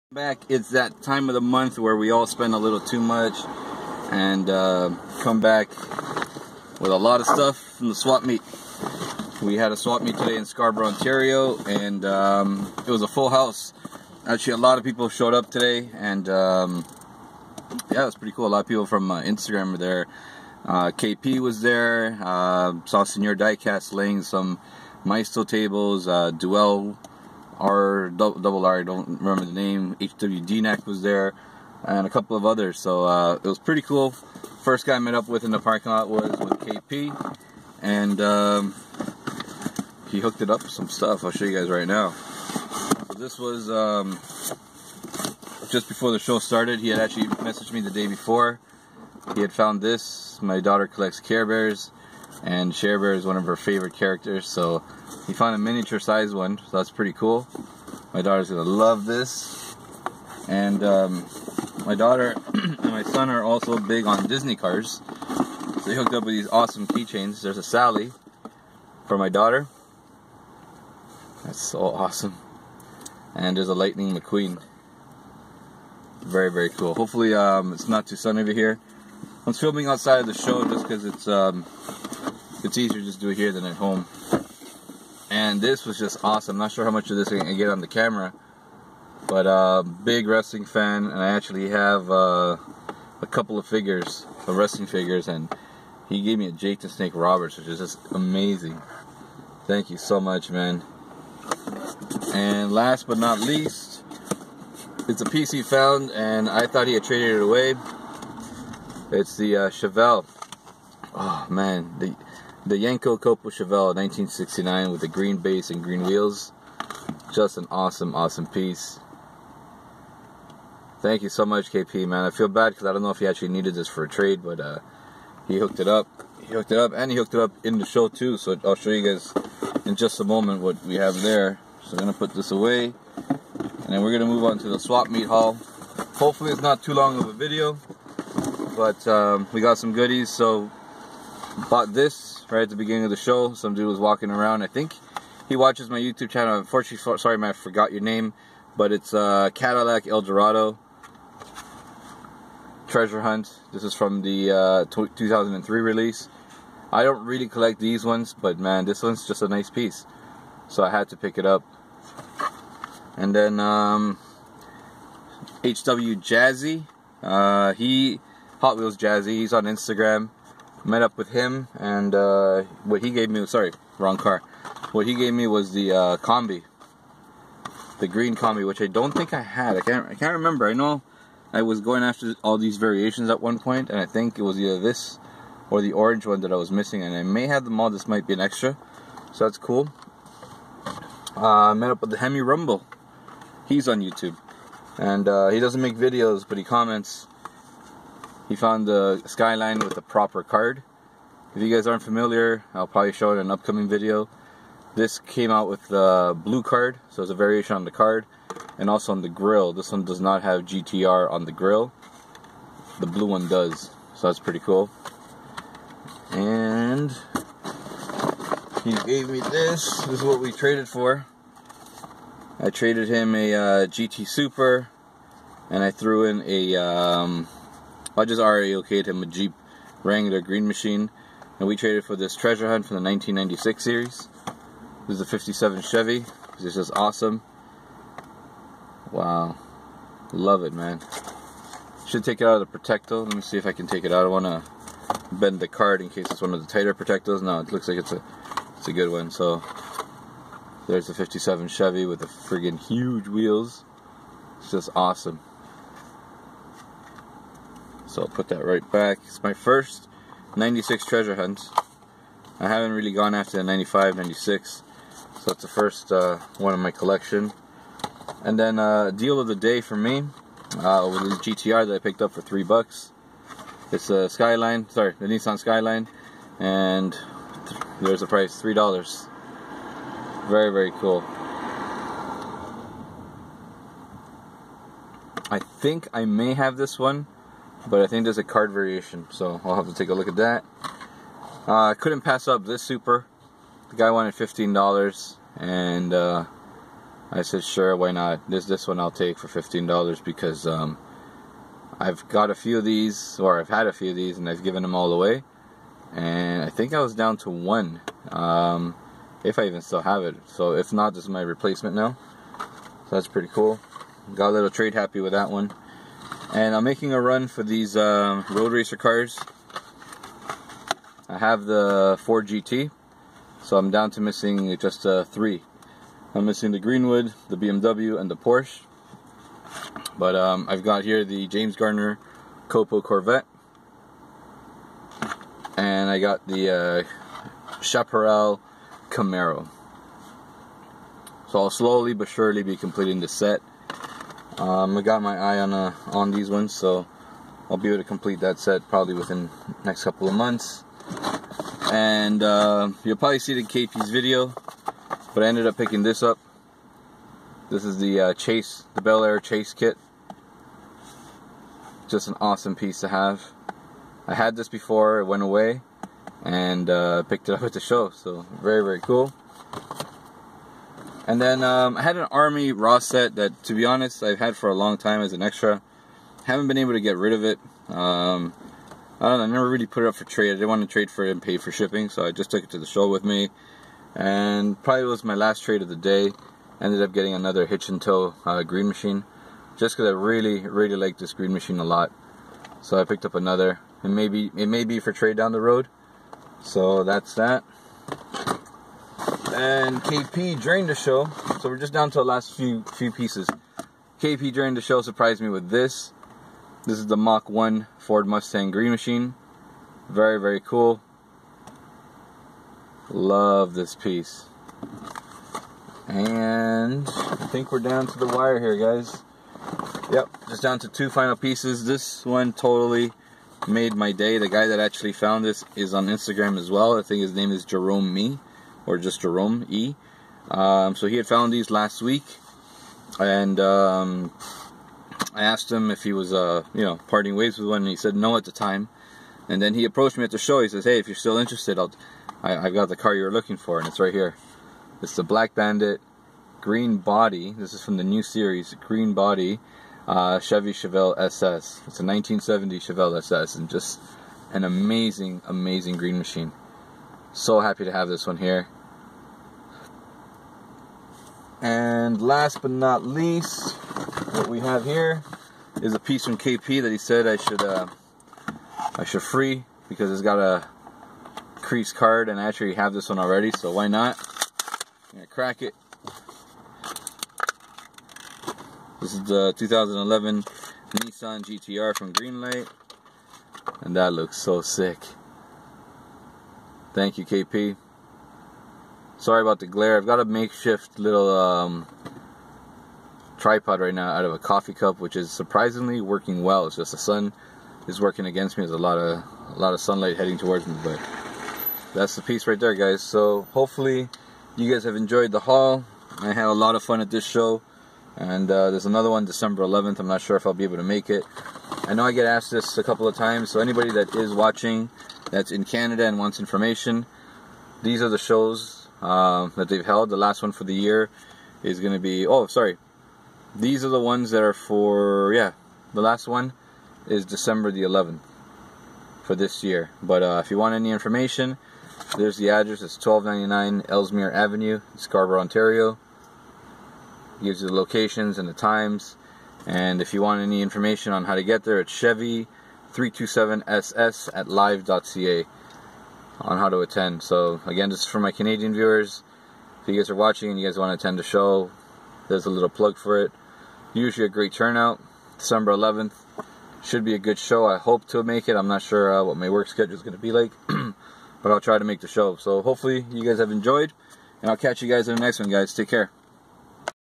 Scarborough, Ontario Diecast Swap Meet. Hot wheels, Greenlight, and M2!
Complete haul from today's Diecast Swap meet at the Birkdale community center in Scarborough, Ontario.